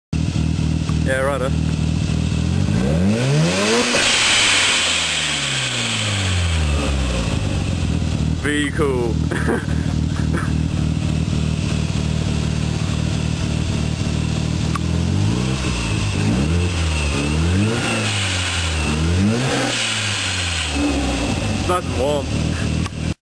My turbosmart dual-port sleeper series BOV
My turbosmart dual-port sleeper series BOV, cars running 11psi making 202rwkW. 13B stock turbo, race extended ports. FC3S.